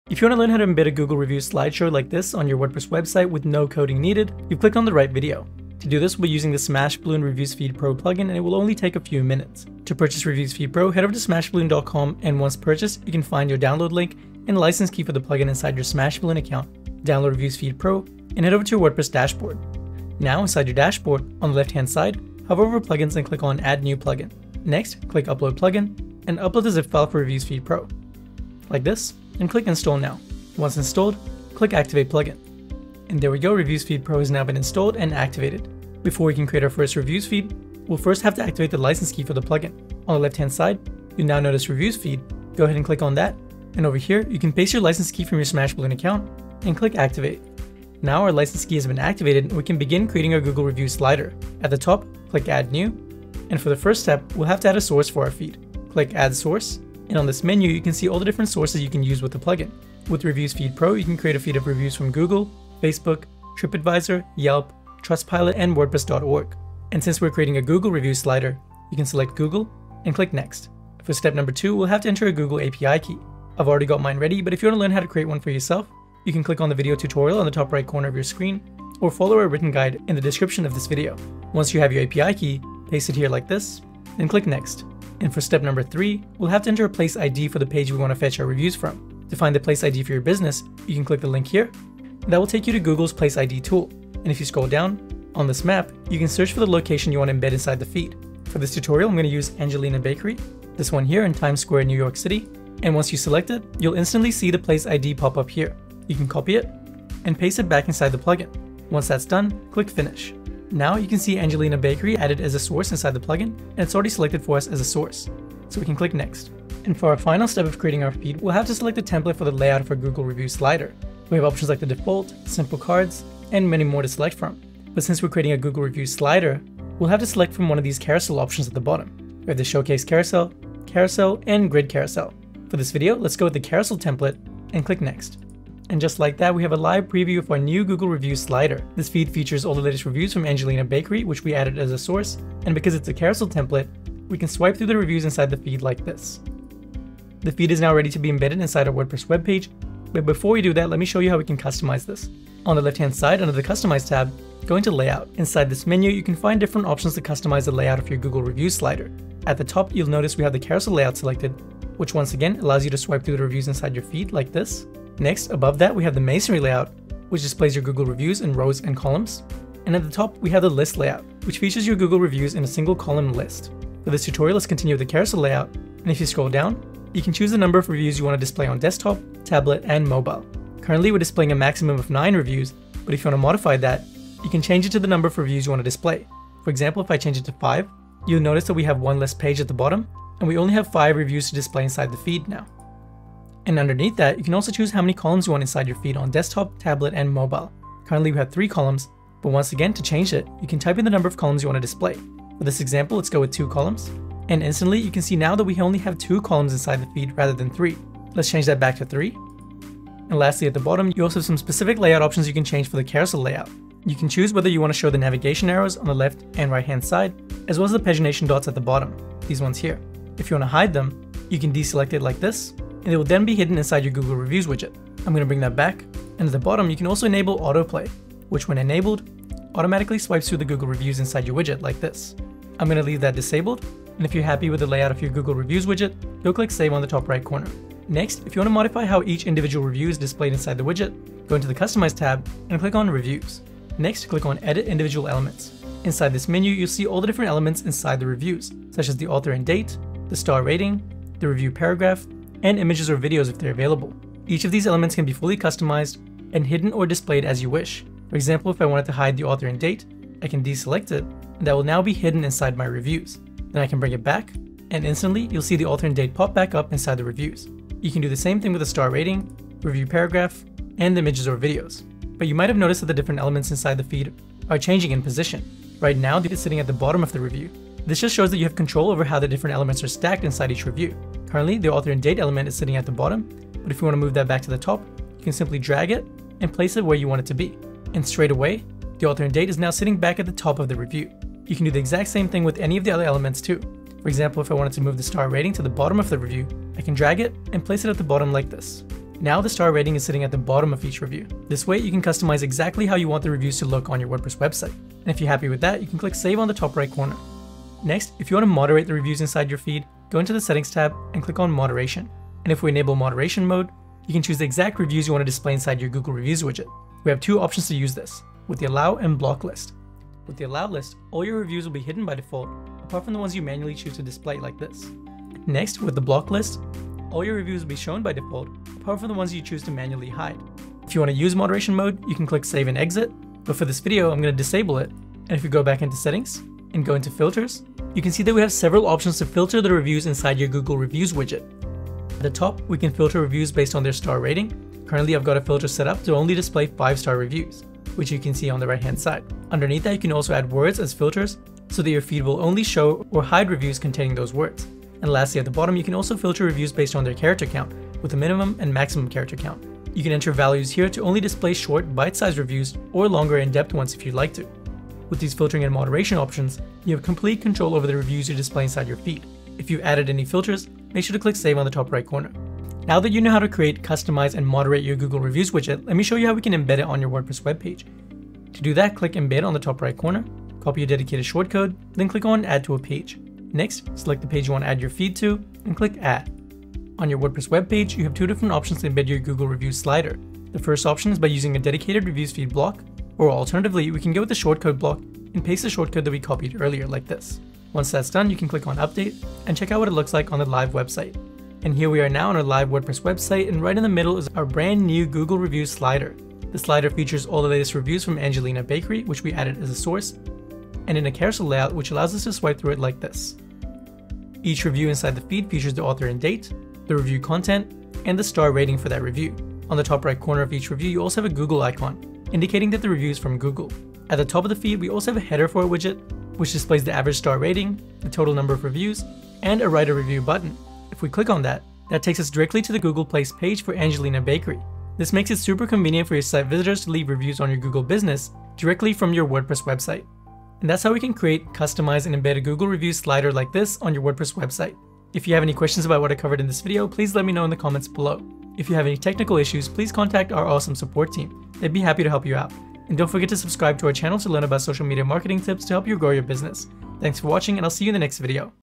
How to Add a Google Review Slider to Your WordPress | Smash Balloon Reviews Feed Pro Plugin
Want to embed a WordPress Google review slider with ease?

As you know, Google is a fantastic source of positive customer reviews. Once you embed these reviews on your slider, you’ll have an easy time boosting your conversion rate and sales.

But if you’re not already familiar with coding, adding Google reviews to your site can be tricky.

The good news? We’ve put together a video tutorial on embedding a Google review slider in WordPress with ease – absolutely no coding needed.

Check out our blog post on How to Add a Google Review Slider to Your WordPress: